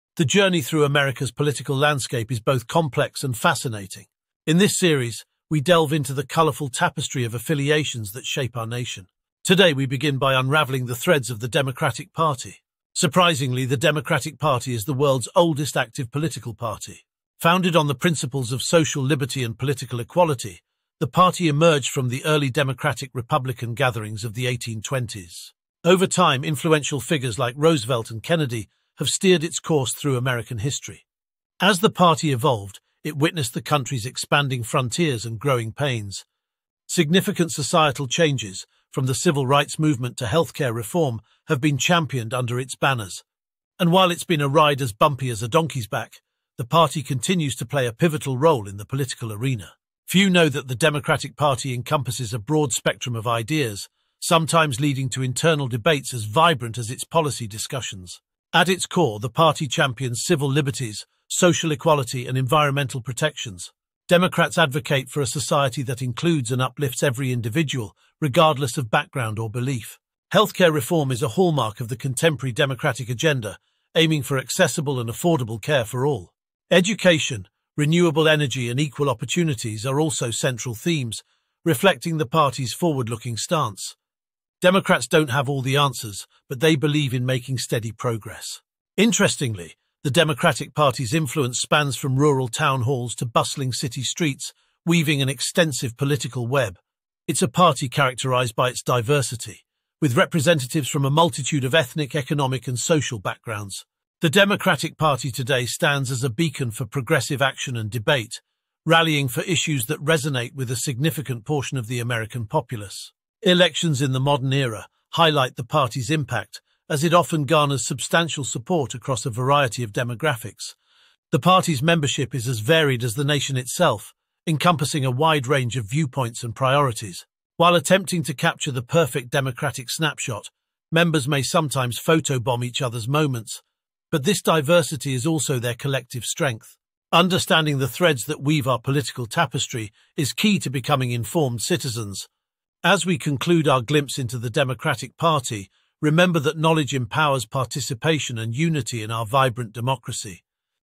What is the Democratic Party? A Journey Through History, Ideology, and Impact
Welcome to our new video series, where we delve into the fascinating world of political parties, starting with an in-depth look at the Democratic Party. This series aims to uncover the rich history, core beliefs, and modern dynamics of one of America's oldest and most influential political parties. Our journey begins with a captivating exploration of the Democratic Party's roots, revealing a little-known fact about its early history. We'll then trace the Party's evolution, spotlighting its founding principles, key historical figures, and the significant transformations it has undergone through American history.

In the second chapter, we dive into the core beliefs and policies that define the Democratic Party today. Be prepared for a surprising statistic that highlights a unique aspect of the Party's policy approach. This segment elucidates the foundational beliefs shaping the Party's platform, discussing in detail the contemporary policies and positions it champions. The third chapter brings us to the current landscape, presenting an intriguing fact about the Party's modern structure and influence. Here, we explore the role of the Democratic Party in today's politics, highlighting its demographic and ideological diversity. Join us as we uncover the layers of history, belief, and strategy that define the Democratic Party.